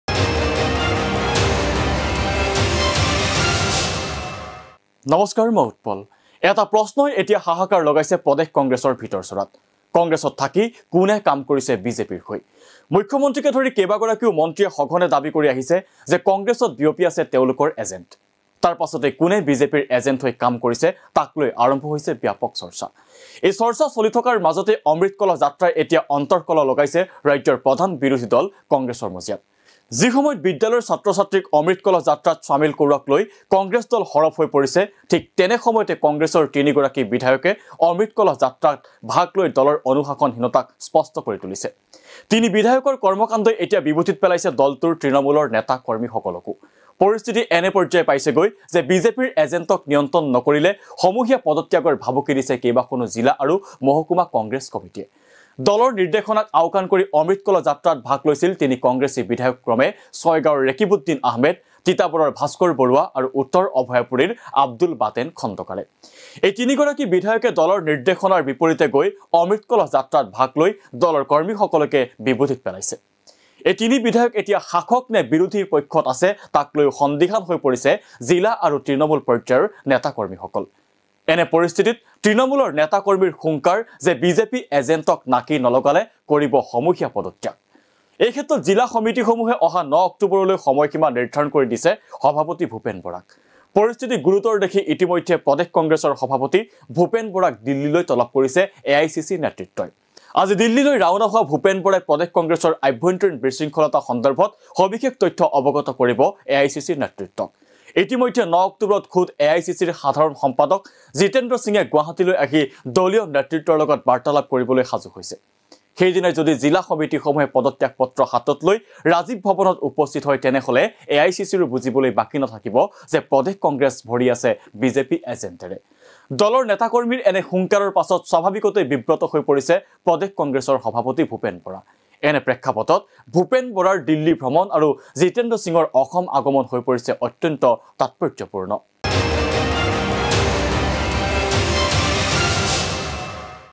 বিজেপিৰ অমৃত কলহে অন্তৰ্কলহ লগাইছে কংগ্ৰেছত!
এটা প্ৰশ্নই হাহাকাৰ লগাইছে কংগ্ৰেছত। দিল্লীলৈ তলব ভূপেন বৰাক। অসমলৈ জিতেন্দ্ৰ সিং। পলিটিক্স অব দ্য ডে--